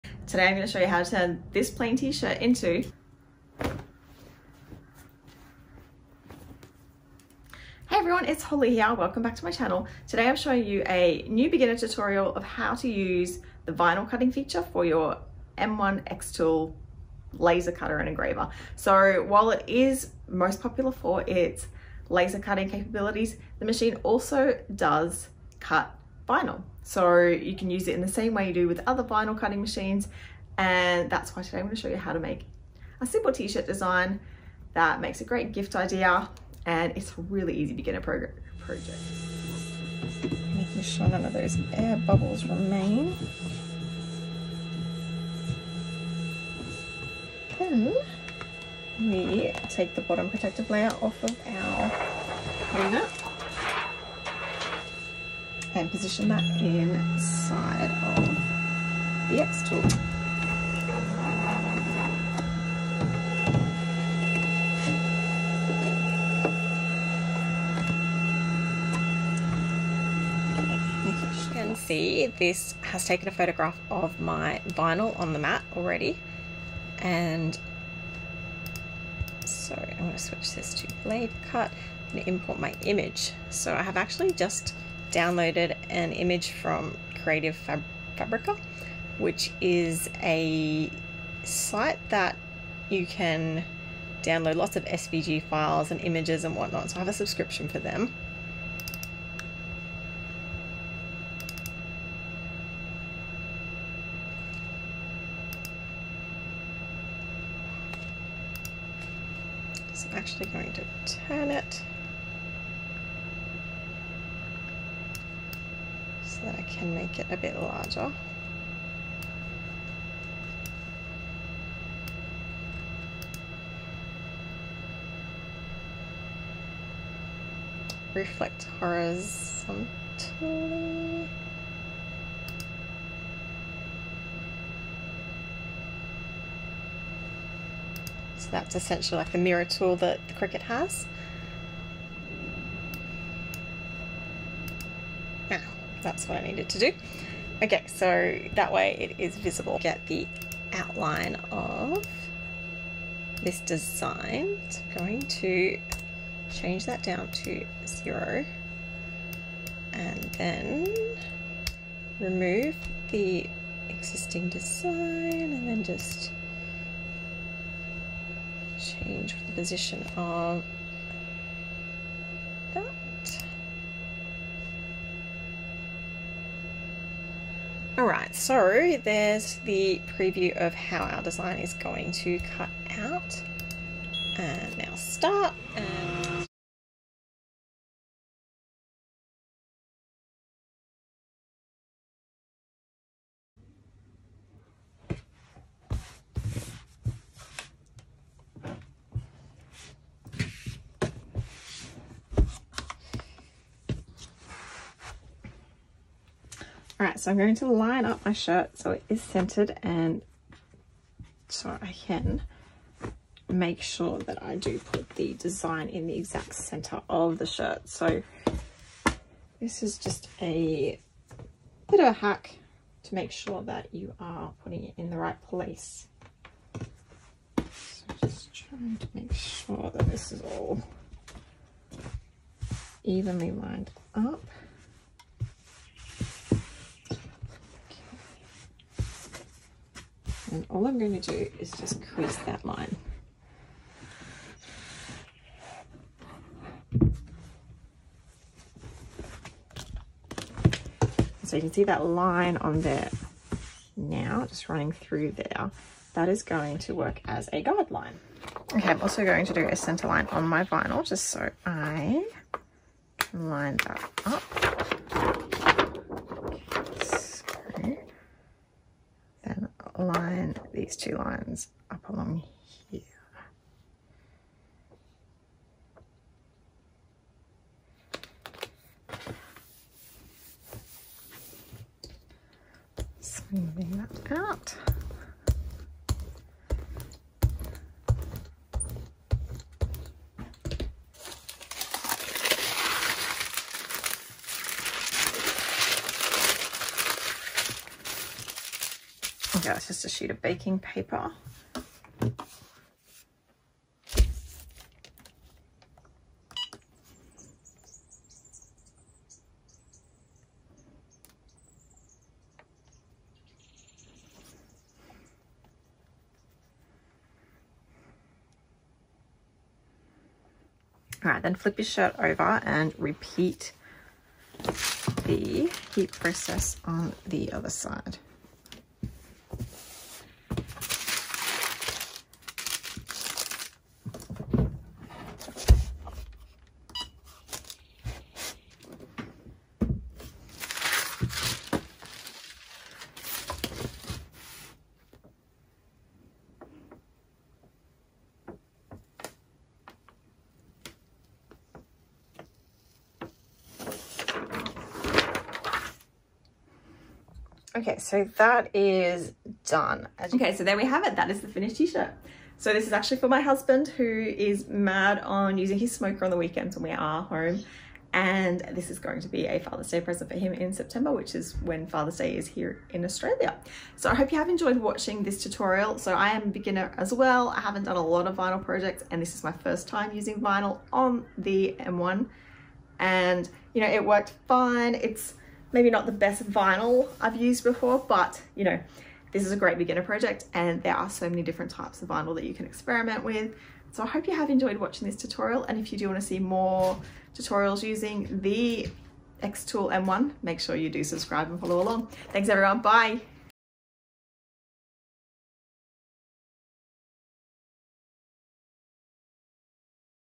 How To Make A Heat Transfer Vinyl Shirt With Xtool M1 Laser & Cutting Machine
Learn how to make a Heat Transfer Vinyl shirt with Xtool M1 Laser & Cutting Machine. This hybrid laser engraving and cutting machine also has a blade so it cuts materials such as heat transfer vinyl just like a Cricut would!

Create a custom shirt with your own design. Using Xtool M1 laser & cutting machine, you can quickly and easily make a high-quality heat transfer vinyl shirt!

I share my beginner xTool project showing you how to do exactly this! From setting up the machine to vinyl cutting and then transferring the design onto your shirt – I guide you through every step.

Make your own unique shirt using Xtool M1!

To learn more about the xTool M1:
Raed my xTool M1 review:

👉 Get my FREE 5 day Deep Clean challenge in your inbox